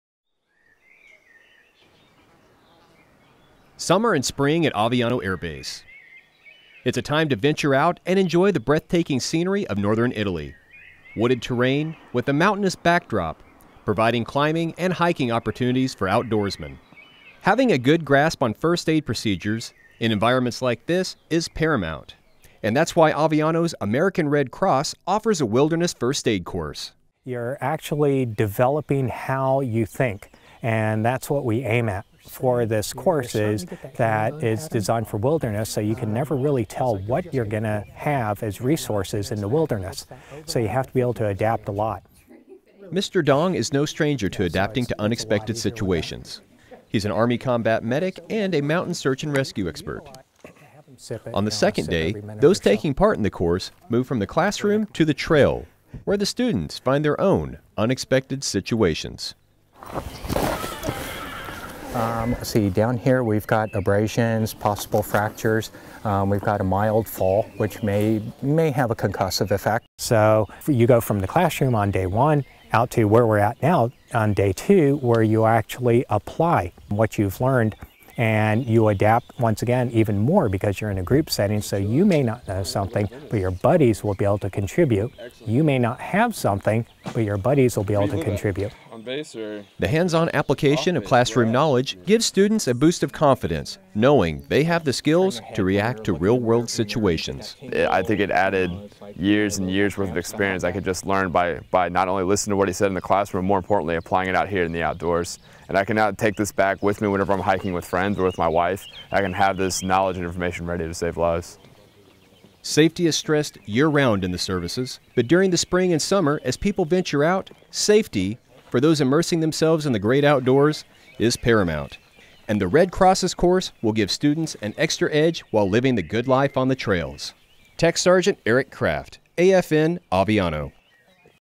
Wilderness First Aid
Warmer weather is on its way to Italy, which means nature lovers will be hitting the mountains around Aviano Air Base. TSgt Eric Craft shows us a few hikers that are taking steps to ensure they arrive back home safely, after a day in the great outdoors.